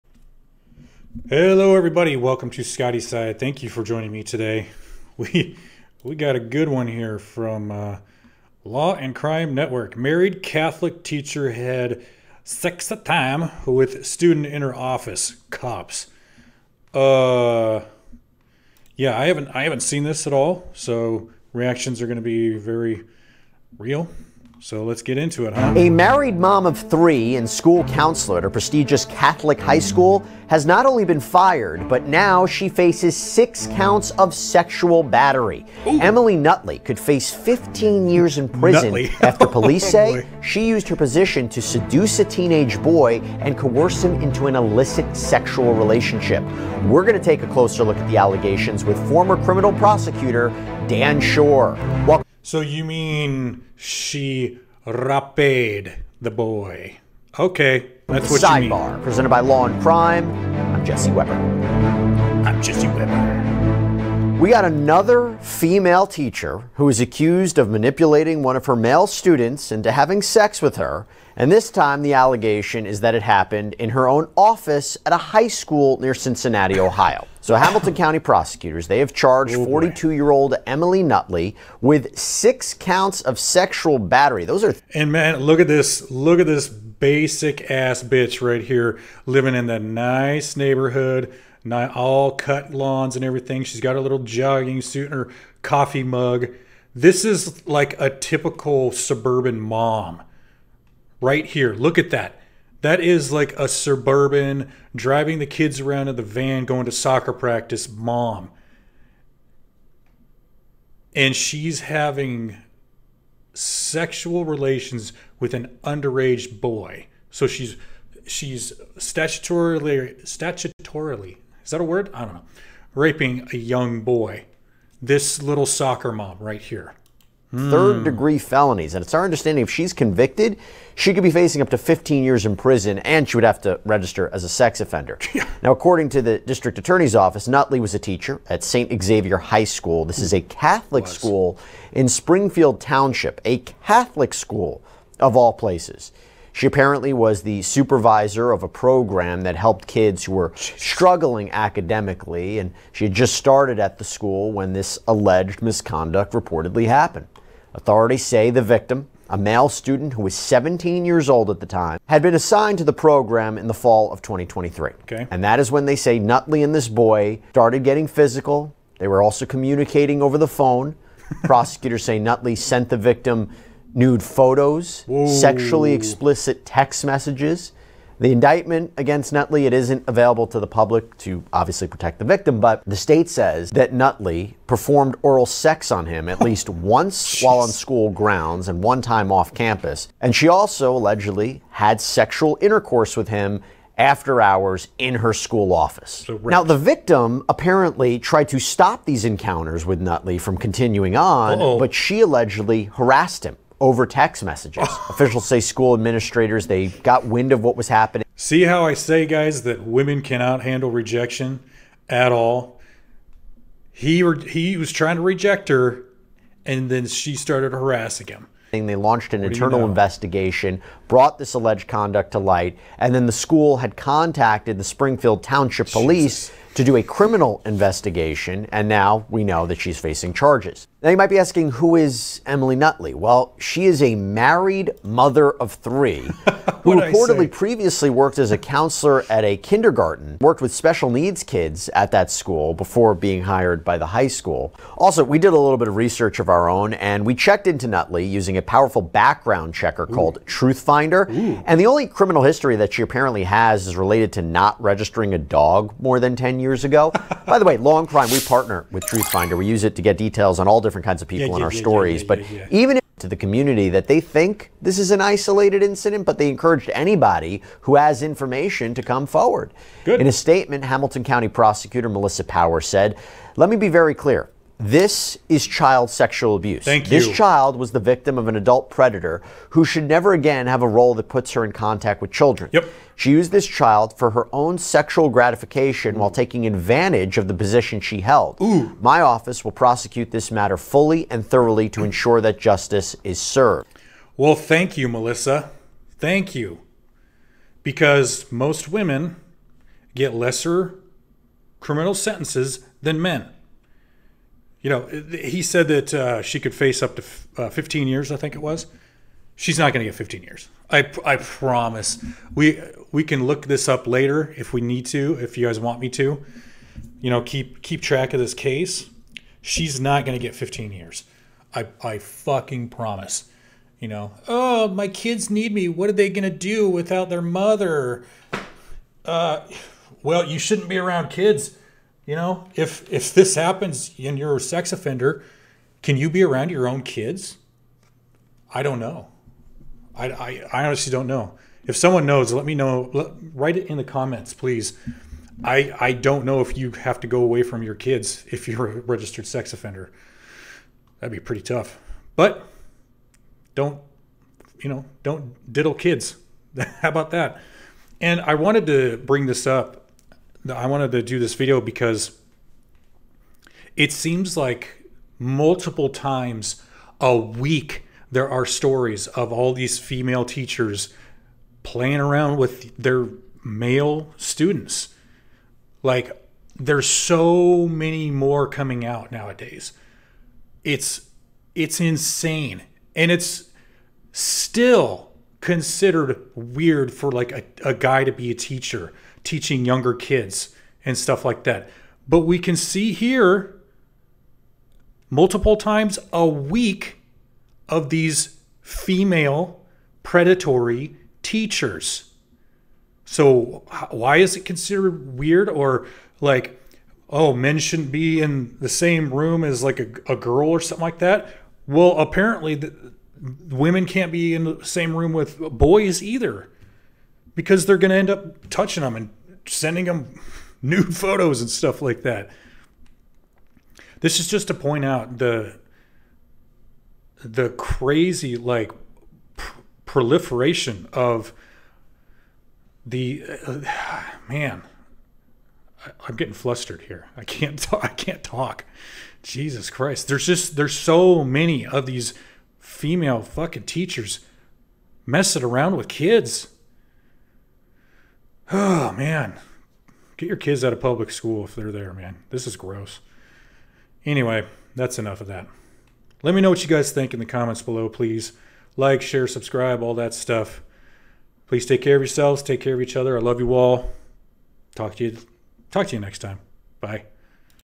[Response] Married, Catholic Teacher Mom SA'ed Boy.
Yikes... yet another teacher fiddling with children.